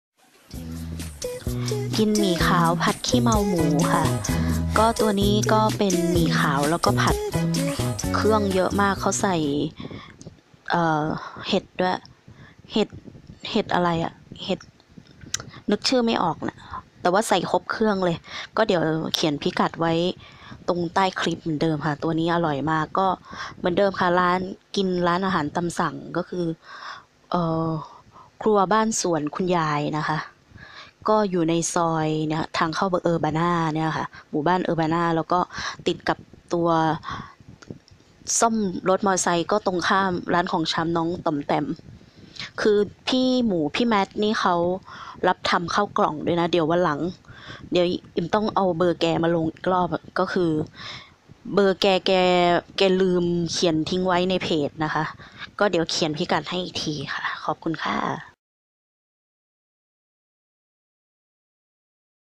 กิน หมี่ขาวผัดขี้เมาหมู จ้ะ
พิกัด เชียงใหม่
เลี้ยวเข้าซอยตำหนักใหม่
ผ่านหมู่บ้านเออบาน่า ตรงไป 400 เมตร
ร้านครัวบ้านสวนคุณยาย ด้านขวามือ ติดร้านของชำค่ะ